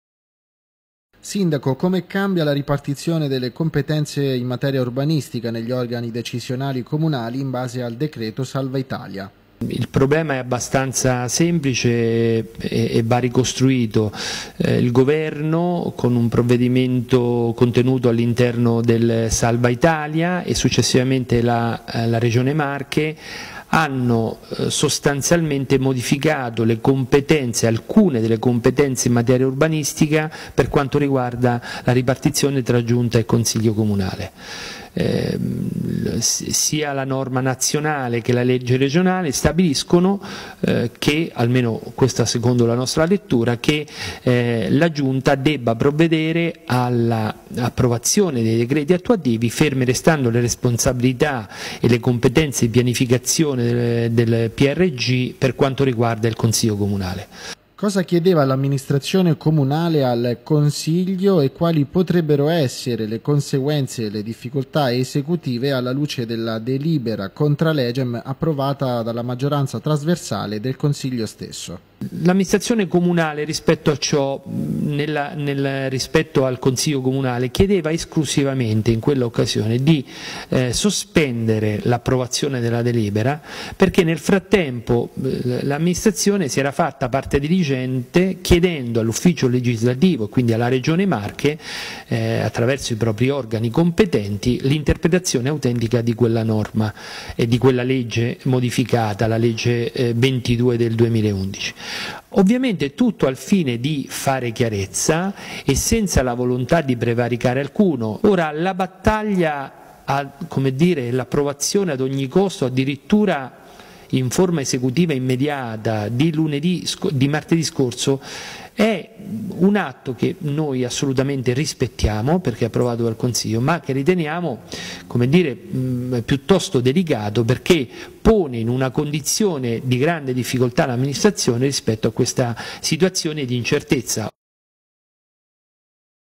MACERATA. IL SINDACO CARANCINI CHIARISCE SULLA DELIBERA DEL CONSIGLIO
Macerata. 13 settembre 2012. Dopo la delibera approvata dalla maggioranza trasversale del consiglio comunale sulle competenze degli atti urbanistici,  il sindaco di Macerata Romano Carancini chiarisce la posizione della propria amministrazione rispetto all'incertezza legislativa in materia dettata dal decreto Salvaitalia. Servizio di Claudio Ricci.